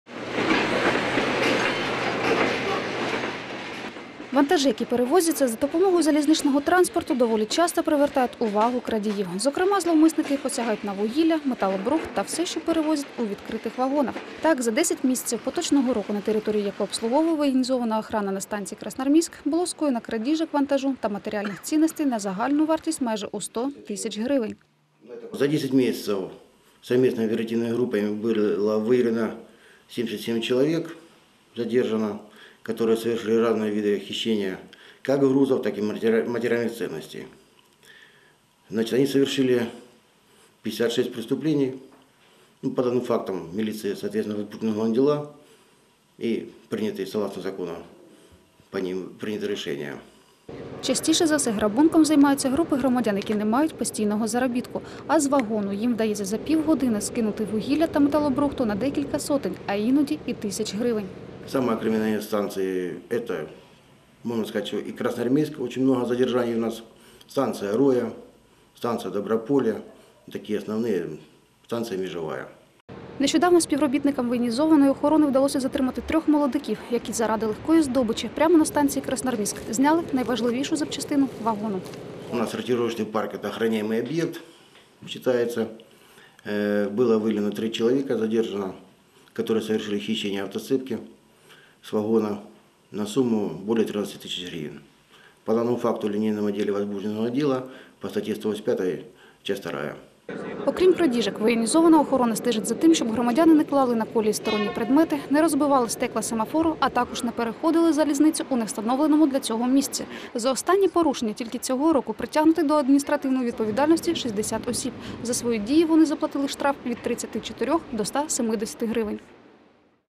Красноармейская железная дорога под надёжной охраной!
За десять месяцев текущего года сотрудниками оперативных групп военизированной охраны Донецкой железной дороги было выявлено 288 случаев краж груза и материальных ценностей на общую сумму свыше 316 тысяч гривен. Свой вклад в общее дело внесли и оперативники стрелково-пожарной команды военизированной охраны станции Красноармейск. Недавно им удалось задержать преступников, которые в буквальном смысле разбирали на запчасти железнодорожные вагоны.